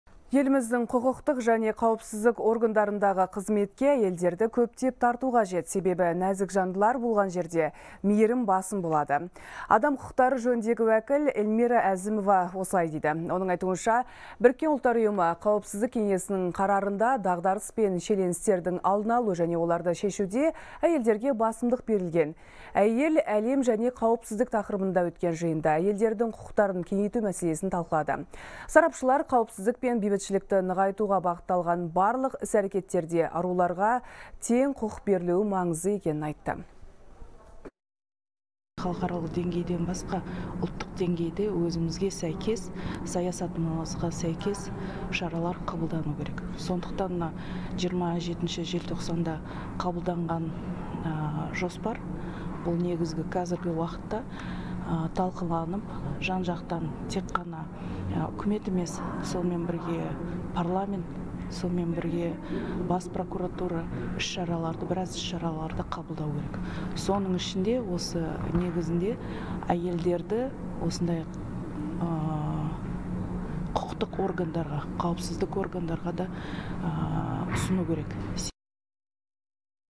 Құқық және қауіпсіздік органдарындағы қызметке әйелдерді көптеп тарту қажет
Еліміздің құқықтық және қауіпсіздік органдарындағы қызметке әйелдерді көптеп тарту қажет. Себебі нәзік жандылар болған жерде мейірім басым болады.